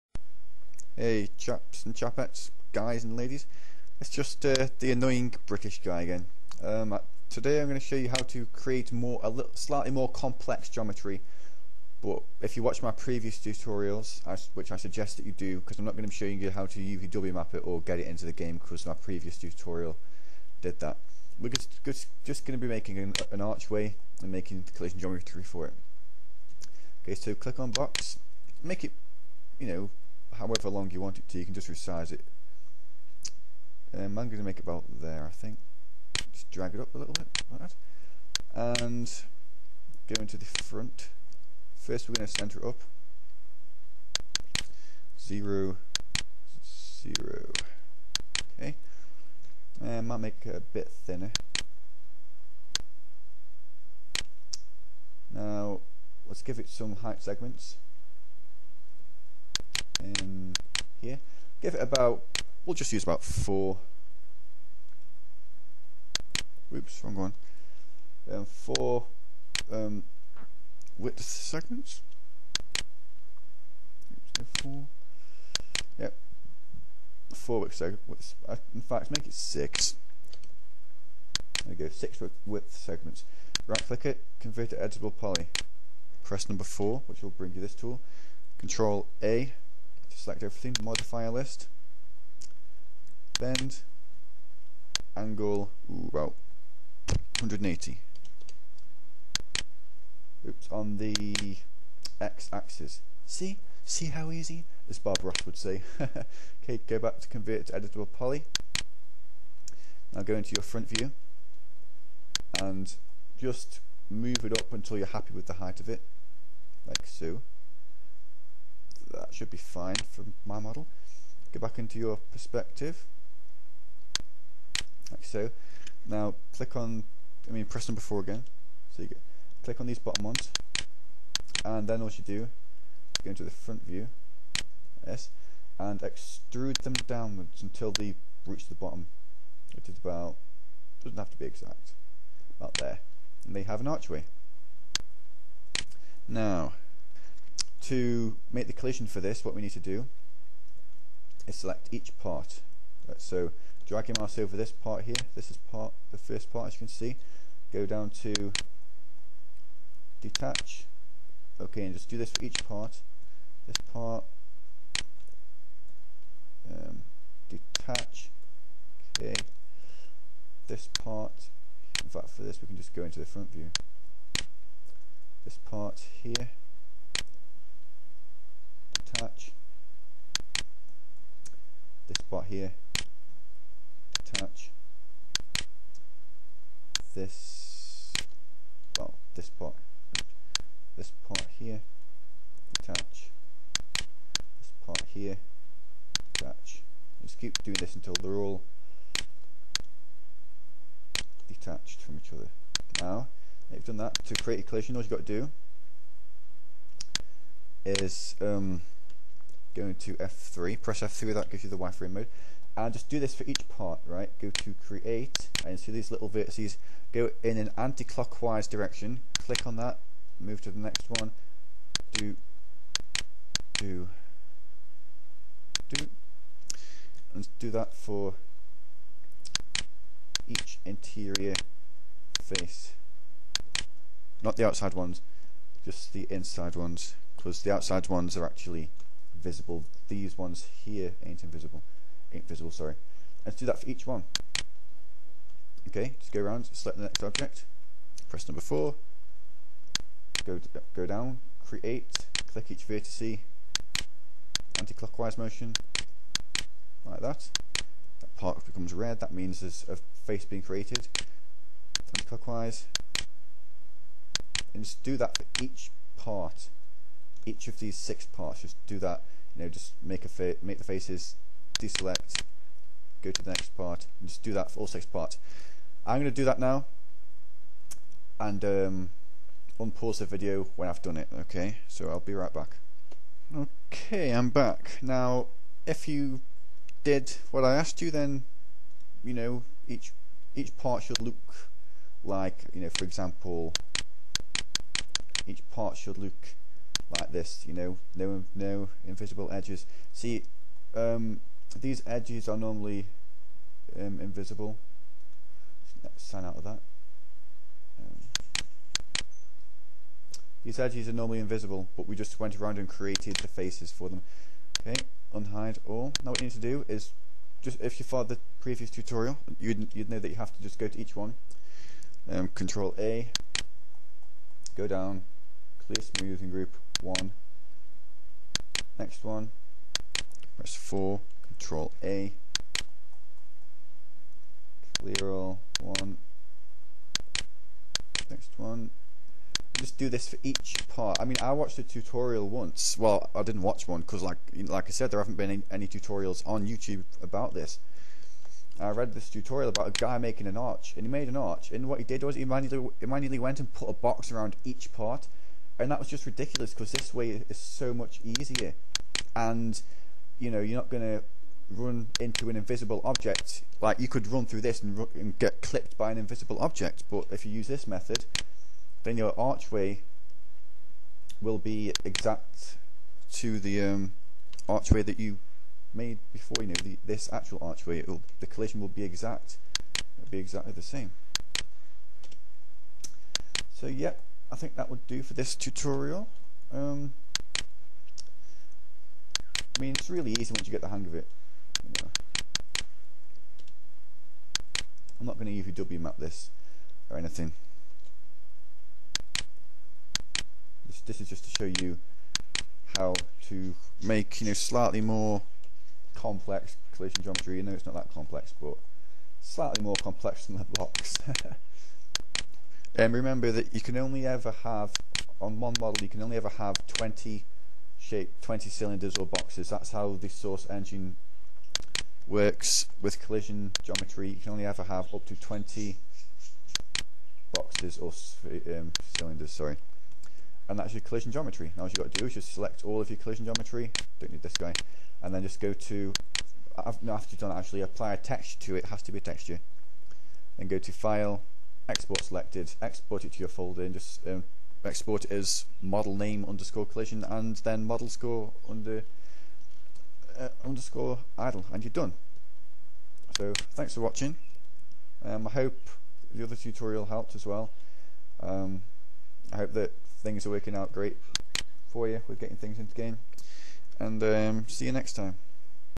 How to make a simple archway for Source/Hammer.
**YOU MUST WATCH MY PREVIOUS TUTORIAL TO LEARN HOW TO EXPORT**
Hi-rez video:

This tutorial explains how to make a simple archway and then shows you how to make the collision geometry for it so you can import it into the Source engine.

I'll not be showing you how to UVW unwrap it though in this tutorial, or how to import it into Source.

If you want to know how to import a prop into Source please watch my previous tutorial.